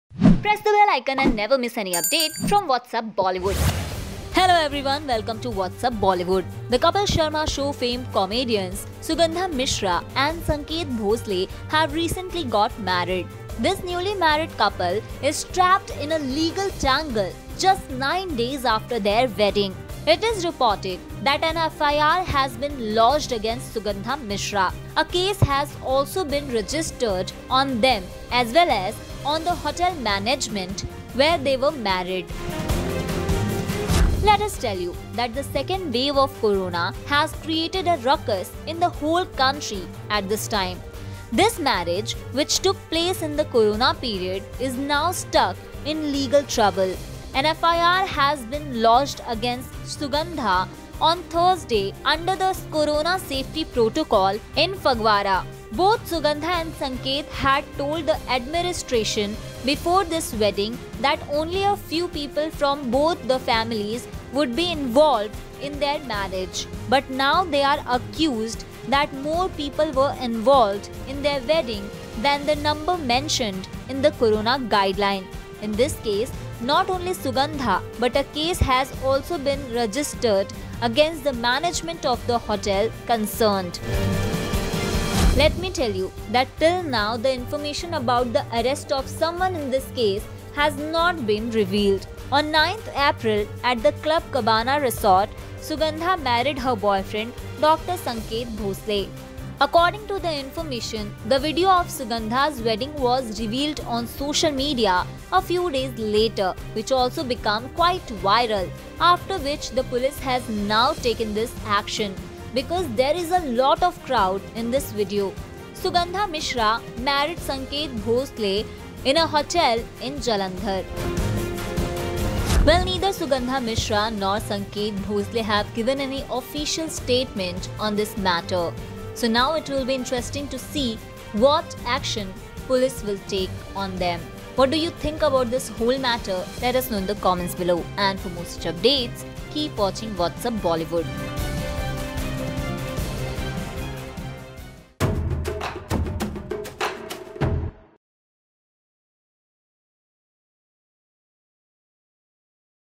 SHOCKING🤐😲Sugandha Mishra , Sanket bhosle Marriage Trouble in 9 Days
The Kapil Sharma Show 'fame comedians Sugandha Mishra and Sanket Bhonsle have recently got married. This newly married couple is trapped in a legal tangle just 9 days after the wedding. It is reported that an FIR has been lodged against Sugandha Mishra. A case has also been registered on them as well as on the hotel management where they were married. Both Sugandha and Sanket had told the administration before this wedding that only a few people from both the families would be involved in their marriage. But now they are accused that more people were involved in their wedding than the number mentioned in the Corona Guideline. In this case, not only Sugandha but a case has also been registered against the management of the hotel concerned.